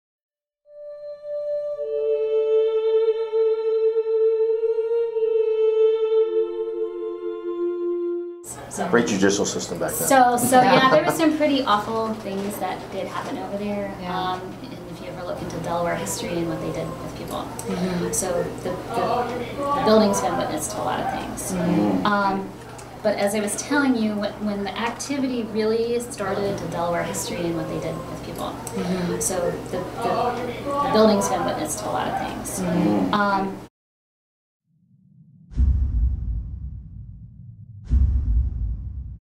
Audio Evidence Penn's Place 1st Floor
Male and Female voices singing.